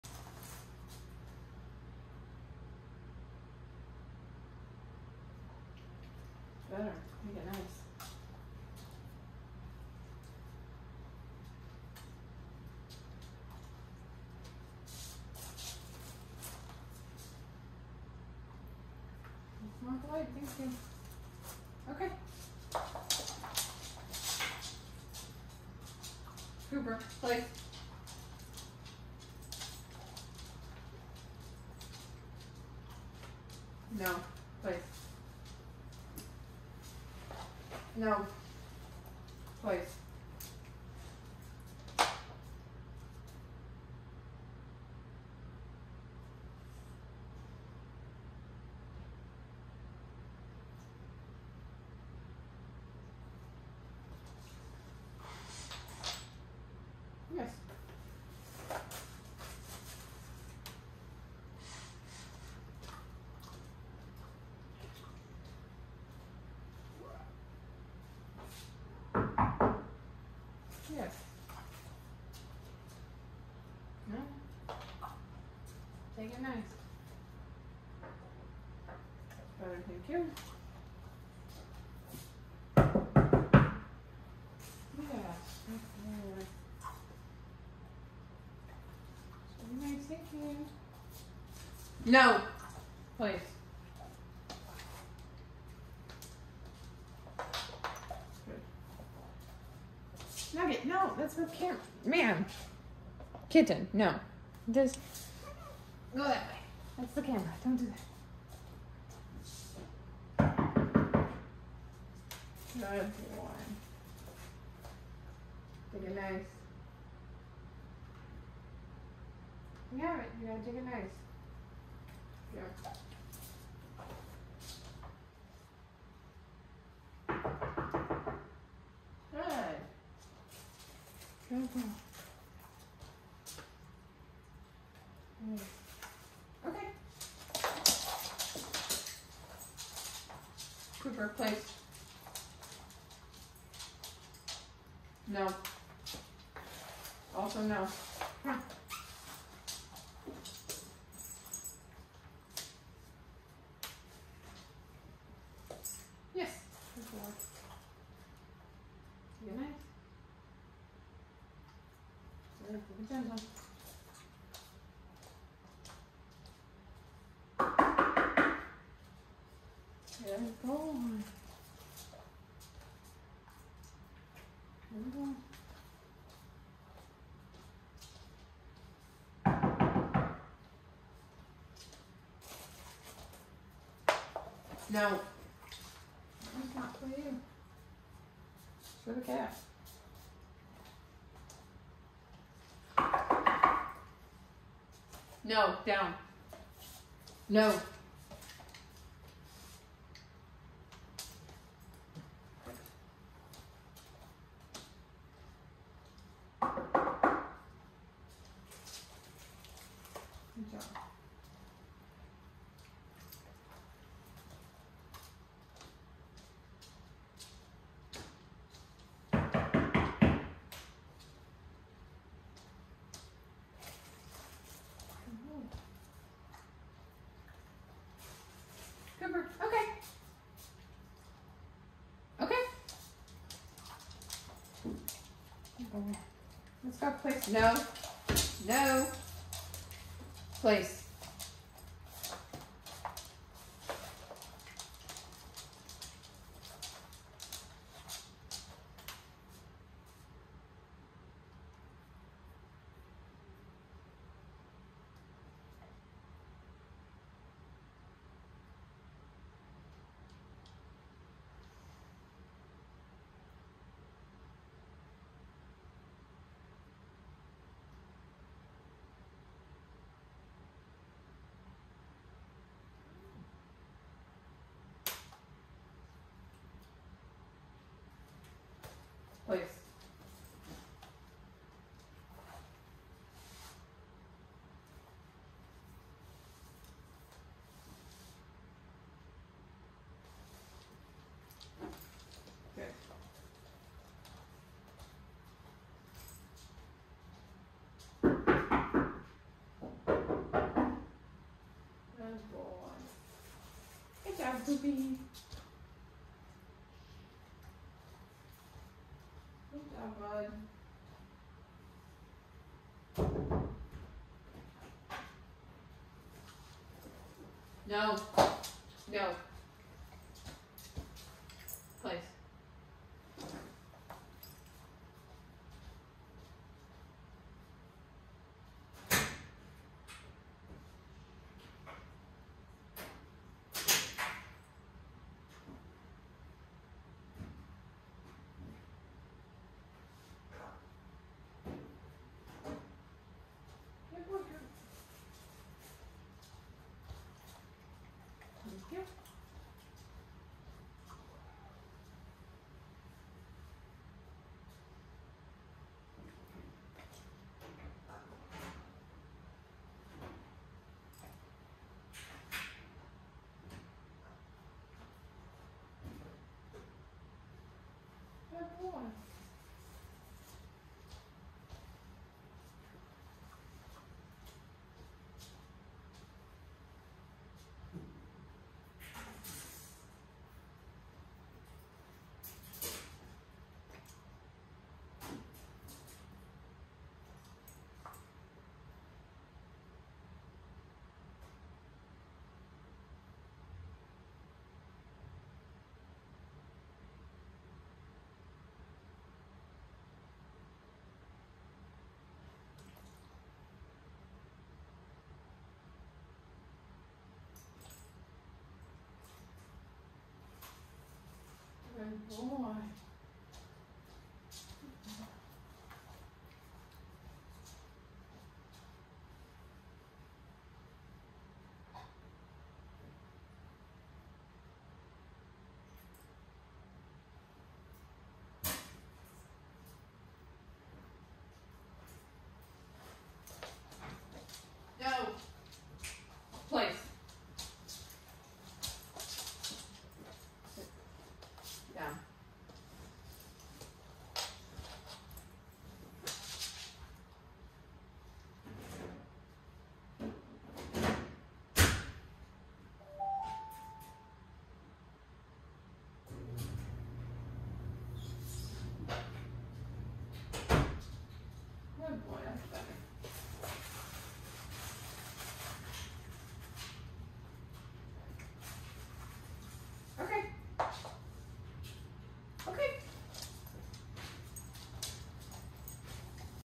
Cooper Day 4 Place
I worked on Cooper's place behavior with knocking distractions.